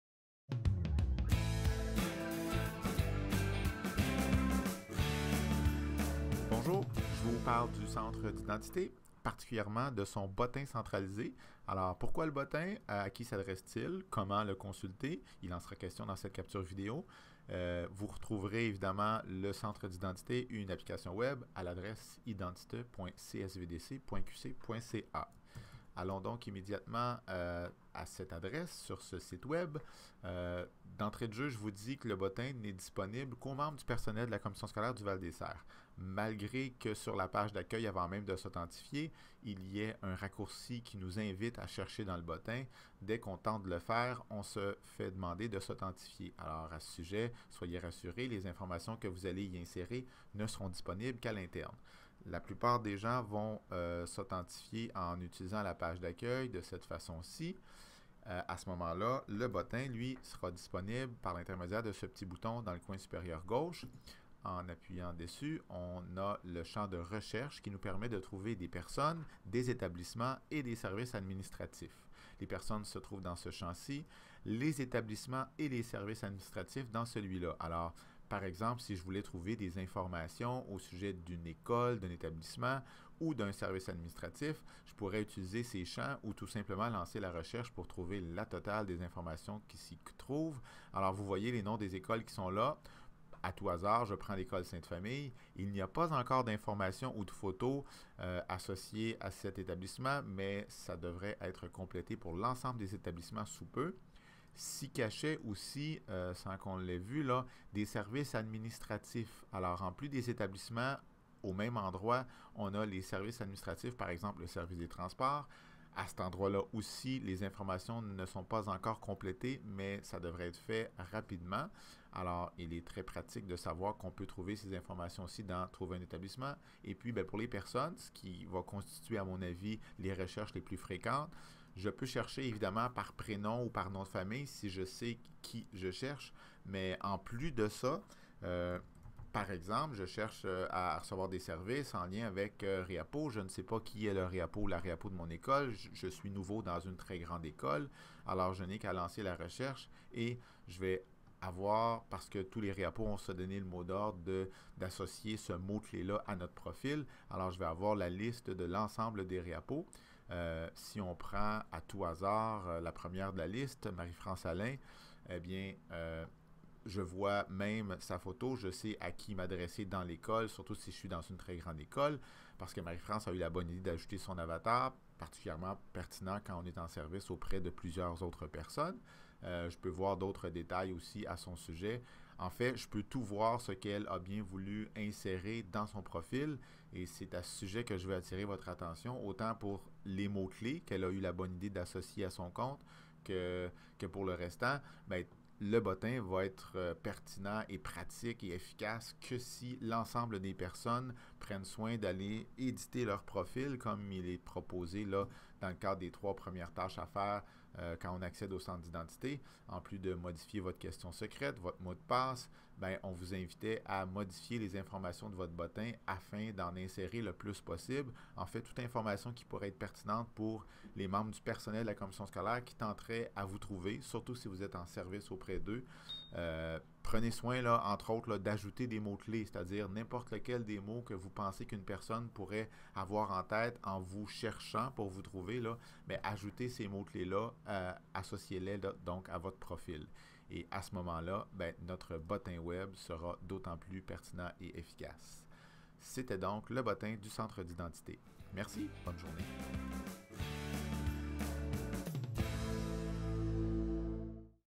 Centre d'identité: le bottin
Comment consulter le bottin du centre d'identité et comment le rendre efficace pour les autres qui nous chercheront